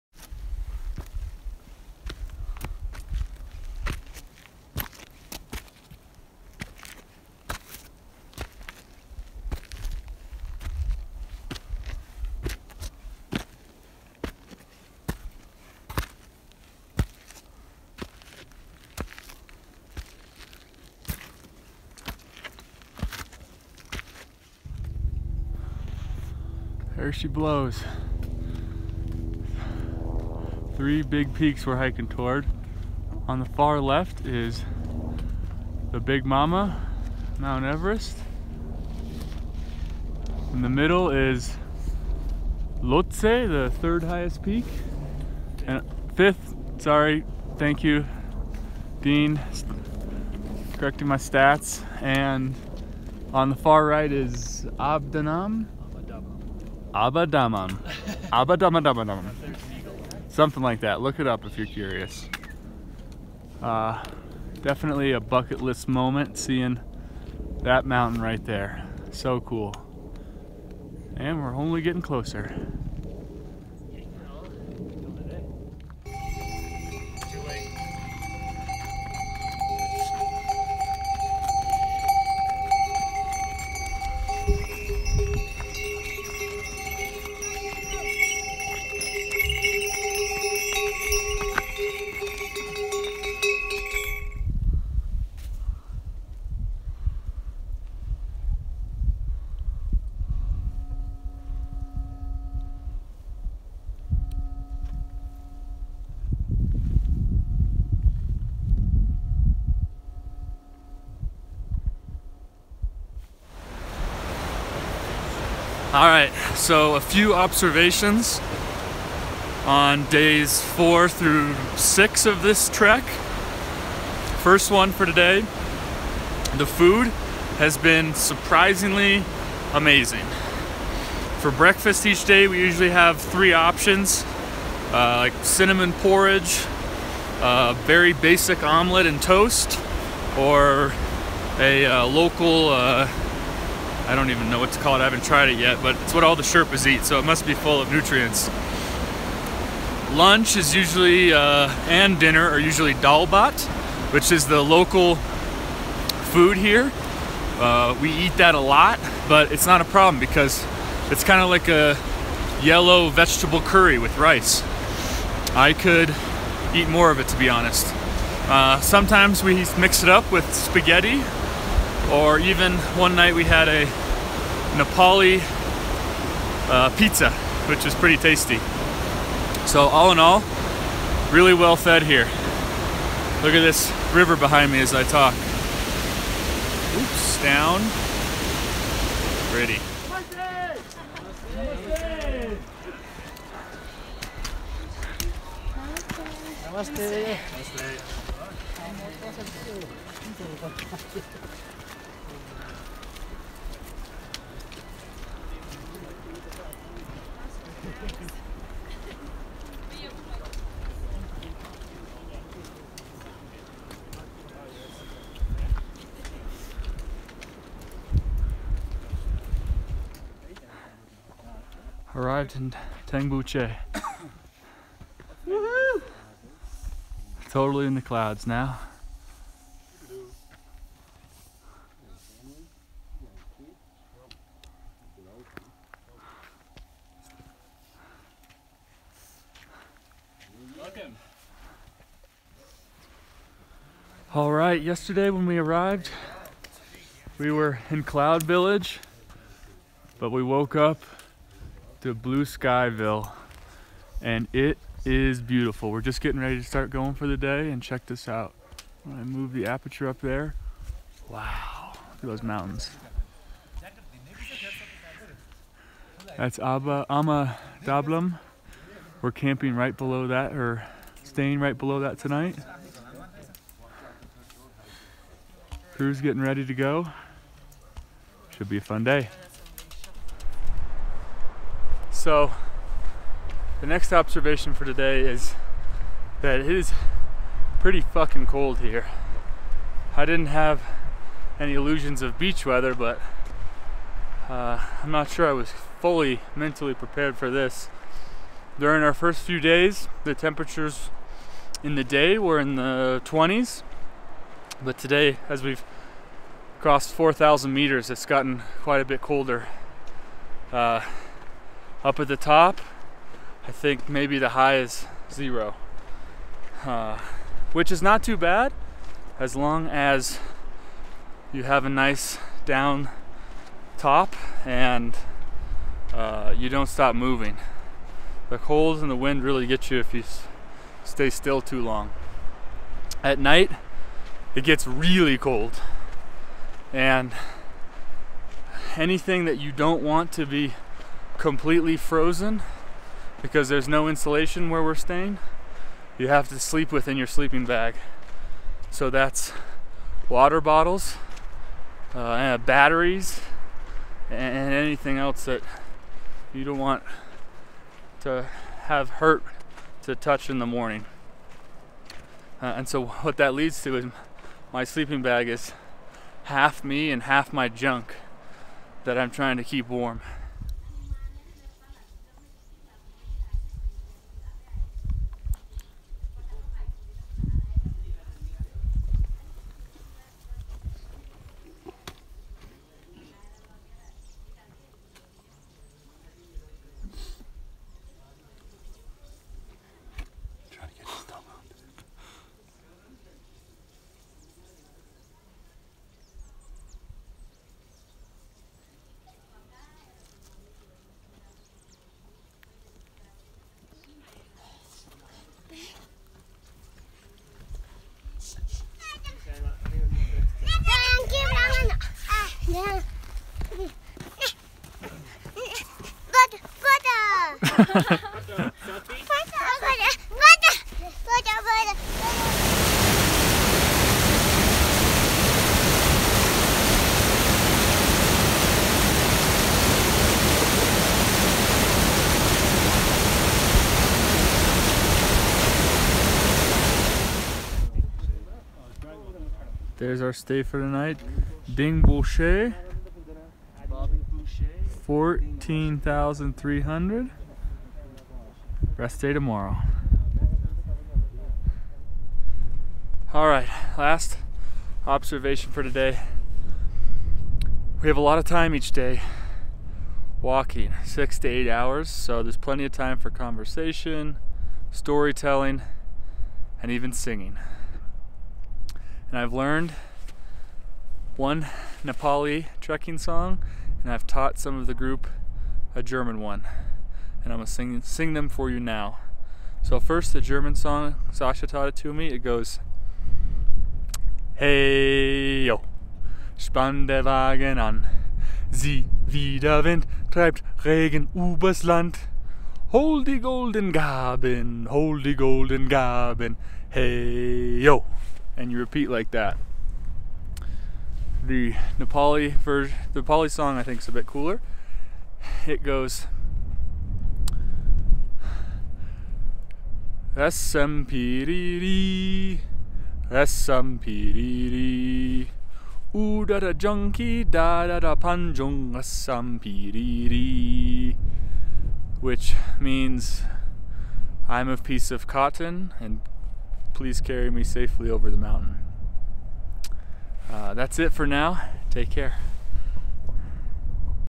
Trekking to Mt Everest Base Camp - Days 4 thru 7
Namche to Tengboche to Denboche with team This World Exists